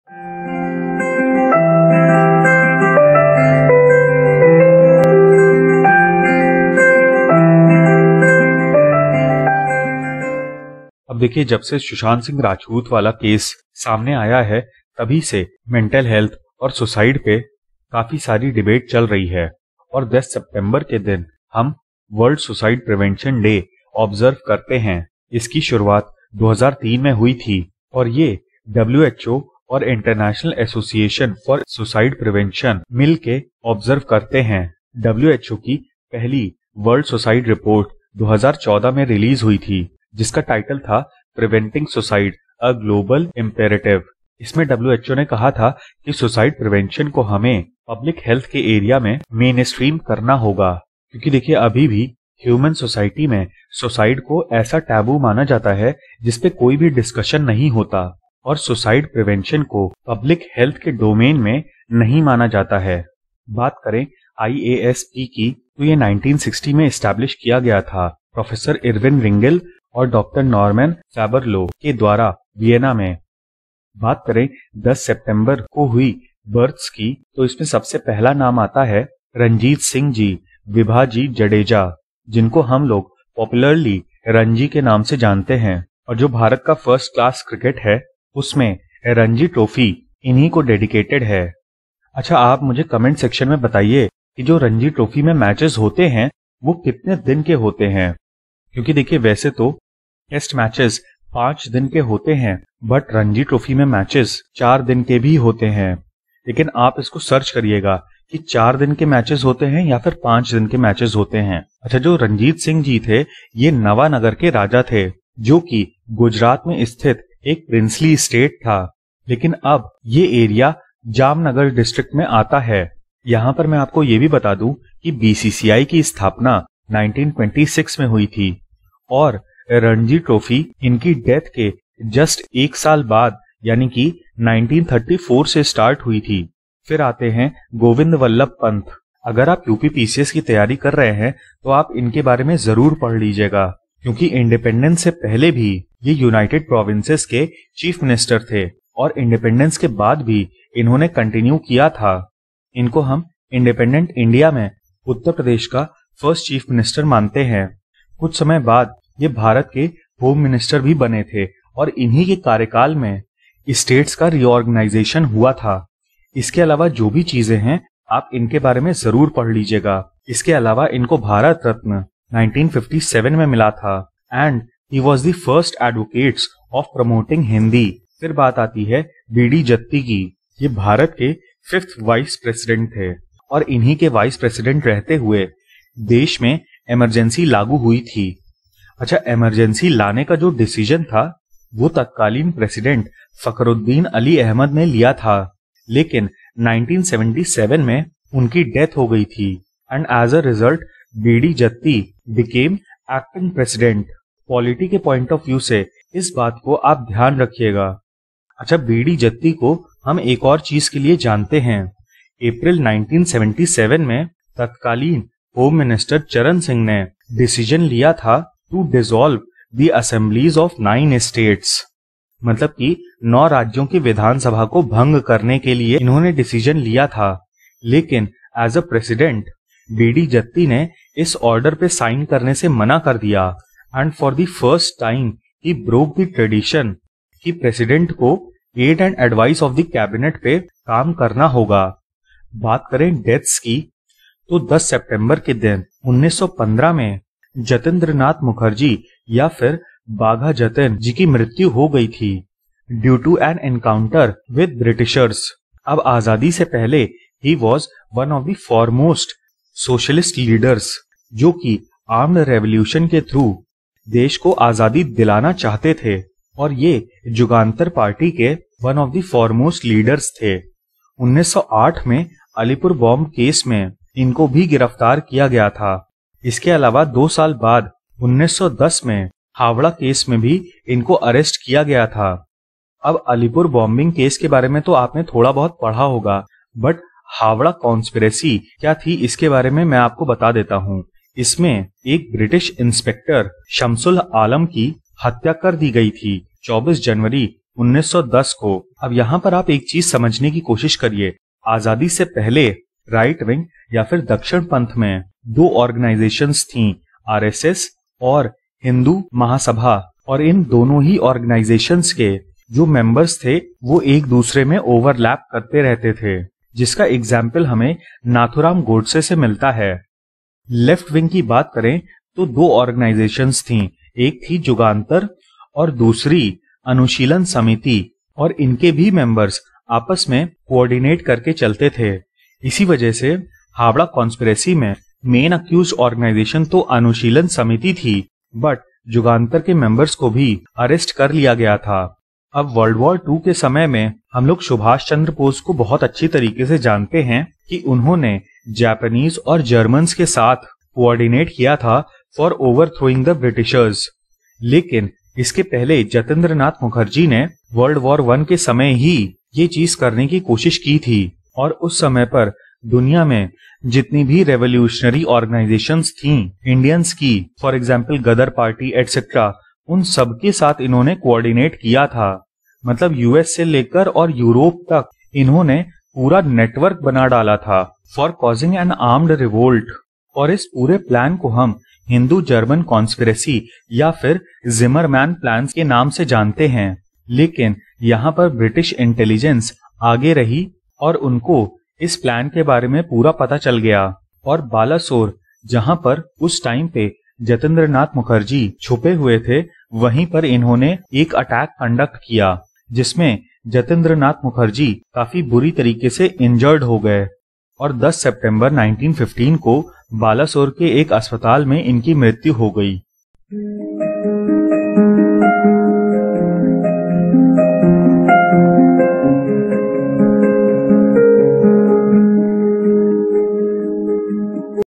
Today in History: 10 September [in HINDI] by GS and Law
Topics covered- World Suicide Prevention Day; In Detail- BD Jatti & Jatindra Nath Mukherjee